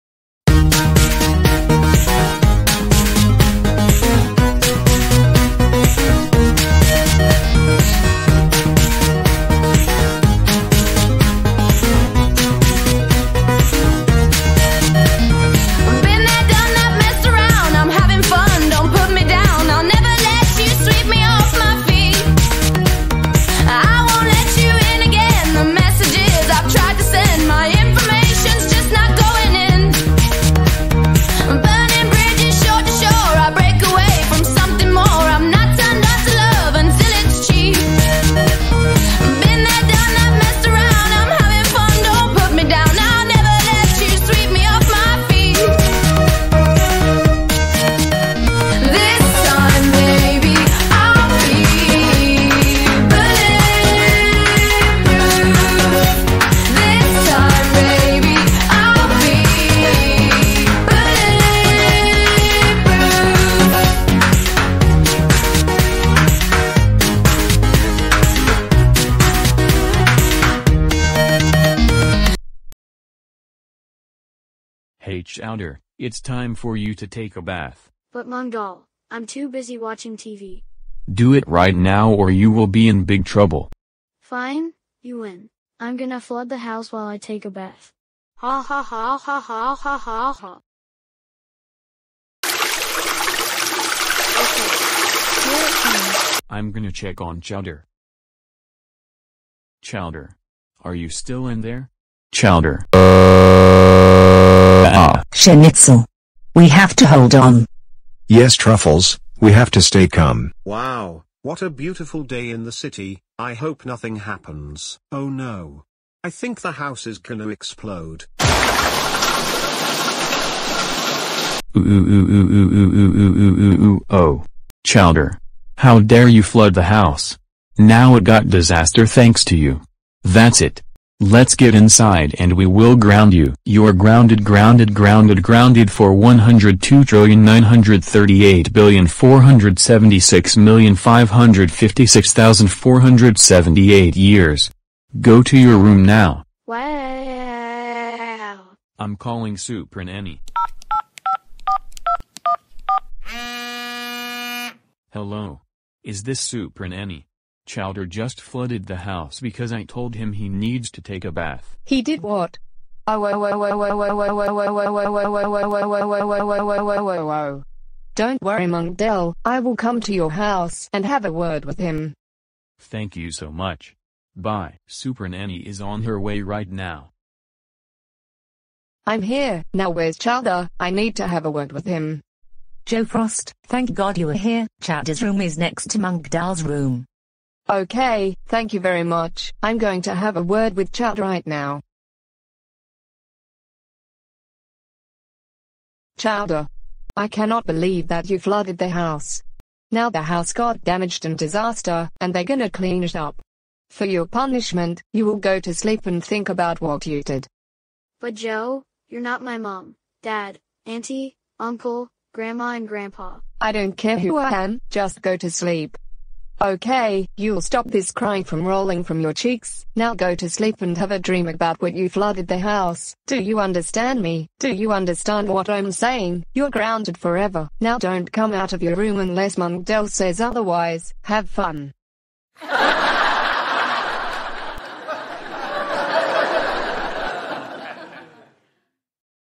Chowder floods the house/Grounded/Supernanny Sent
That's very illegal to flood the house.